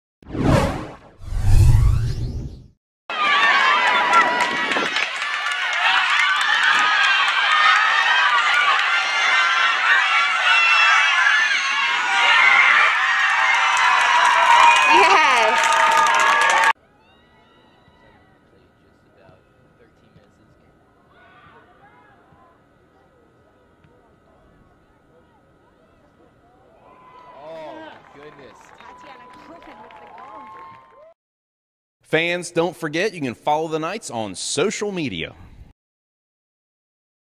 Tatiana Coleman: Player of the Week (9.15.14)
Goal highlights that merited UCF senior forward Tatiana Coleman's second American Athletic Conference Offensive Player of the Week honor on Sept. 15 .